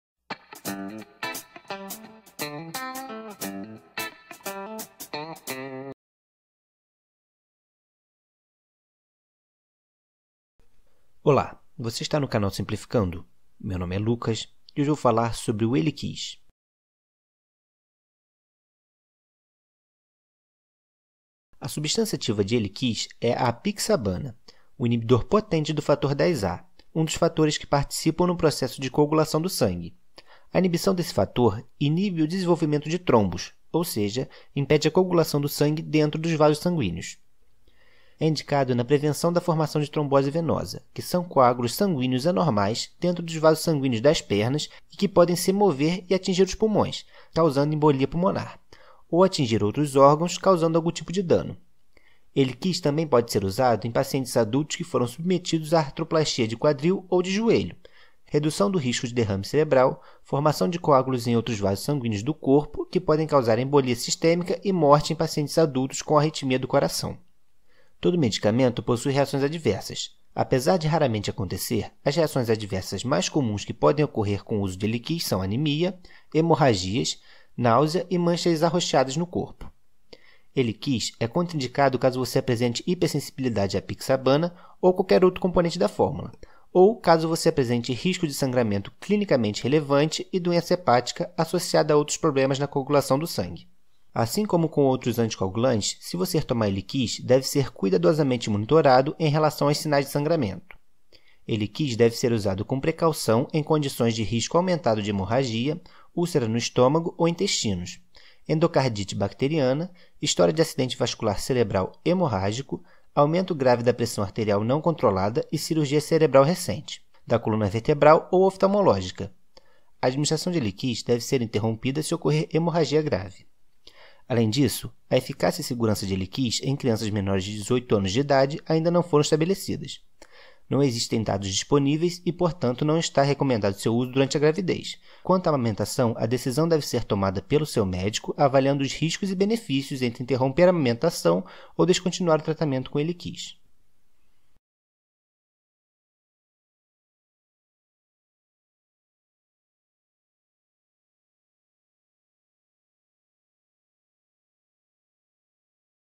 ELIQUIS: Indicações, contraindicações e riscos
A substância ativa de Eliquis é a apixabana, um inibidor potente do fator Xa (um dos fatores que participam no processo de coagulação do sangue). A inibição deste fator previne o desenvolvimento de trombos, ou seja, impede a coagulação do sangue dentro dos vasos.

"ELIQUIS É UM MEDICAMENTO, SEU USO PODE TRAZER RISCOS, LEIA A BULA, EM CASO DE DÚVIDAS, PROCURE UM MÉDICO OU FARMACÊUTICO".